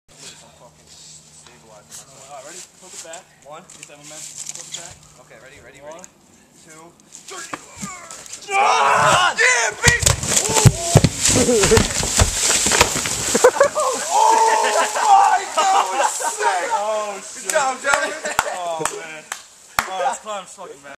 boulder destroys forest
2000 pound boulder rocks a mountain side